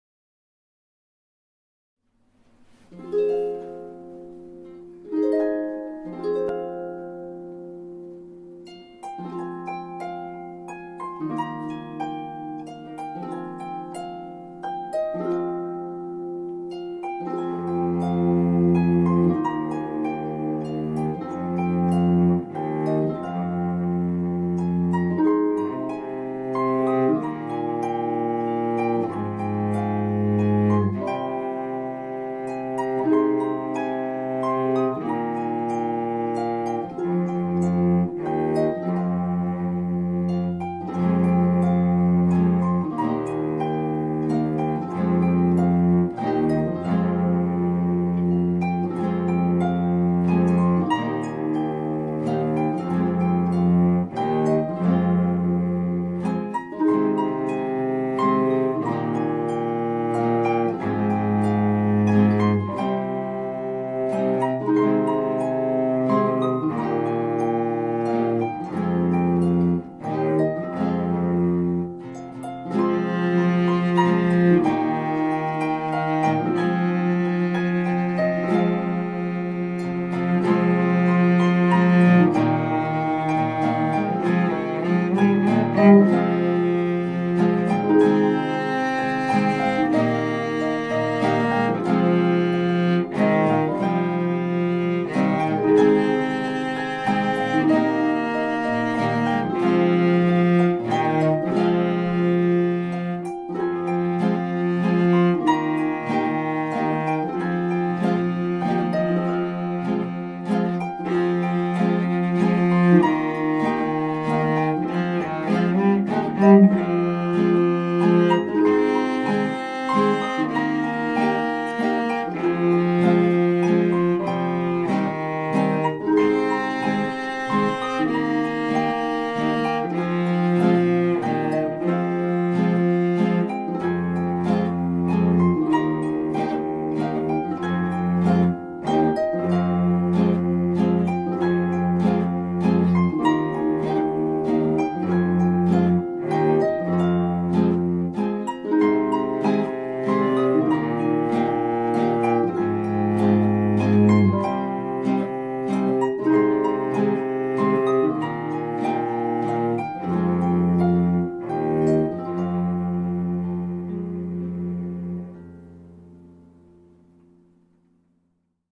Morgane et Mélusine - Mazurka
Jam session with Gregory at the cello - this is a mazurka composed by Mélusine :)